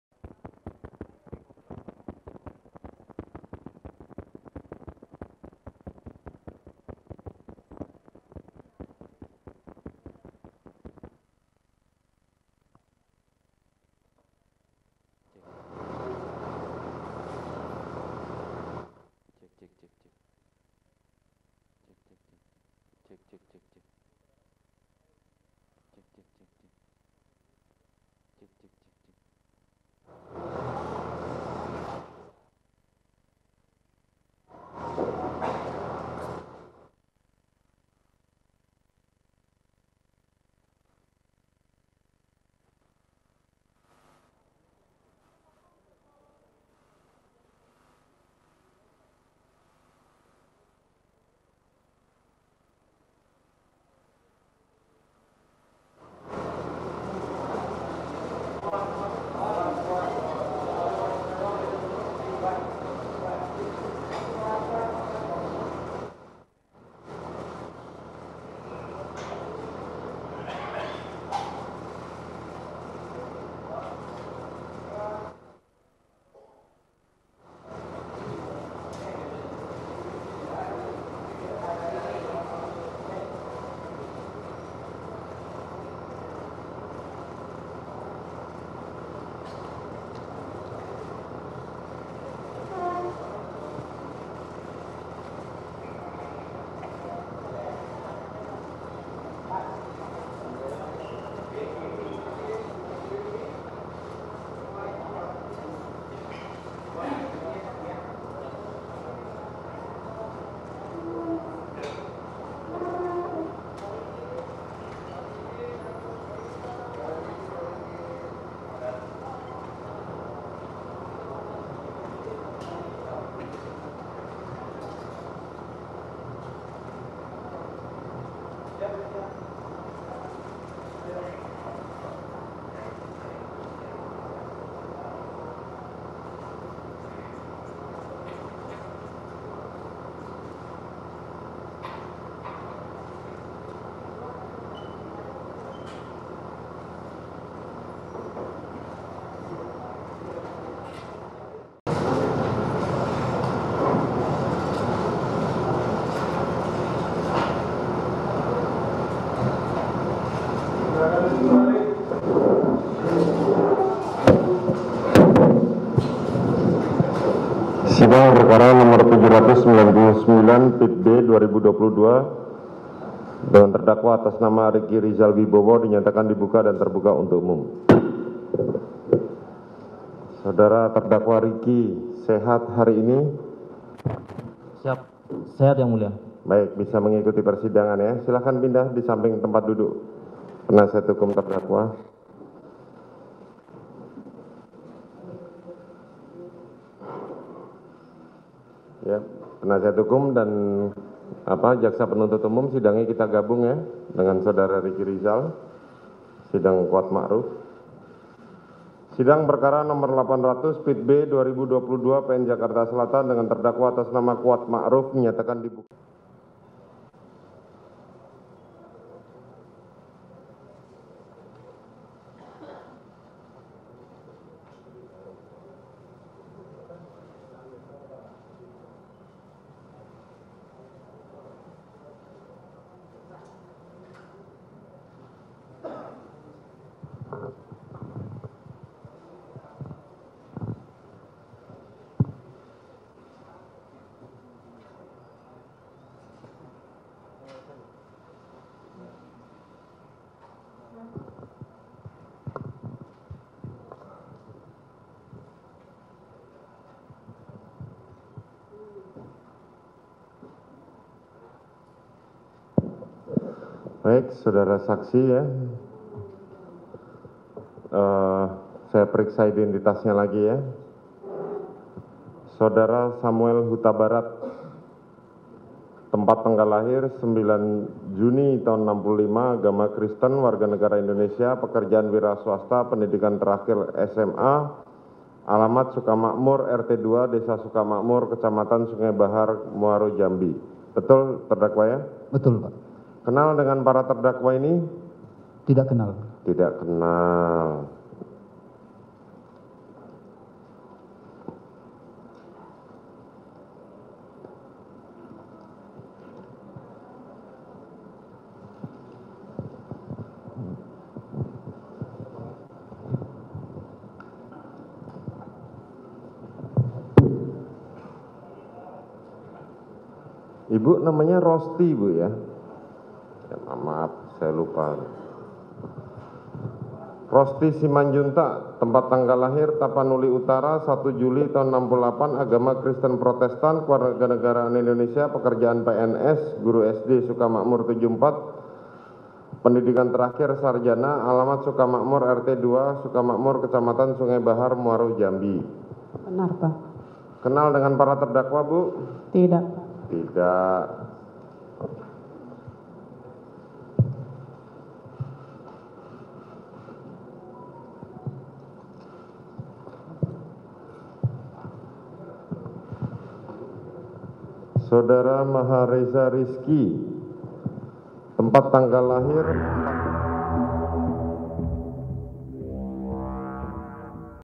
Ekspresi Kuat dan Ricky Bertemu Orang Tua Yosua Pertama Kali di PN Jaksel
JAKARTA, KOMPAS.TV -  Inilah momen pertama kalinya terdakwa Kuat Maruf dan Ricky Rizal bertemu dengan orang tua Yosua pertama kalinya di PN Jaksel, Rabu (2/11/2022).

Keluarga Yosua akan memberikan kesaksian dan menjawab pertanyaan JPU.

Dari sejumlah saksi yang dihadirkan hanya Reza adik Yosua yang mengenal para terdakwa.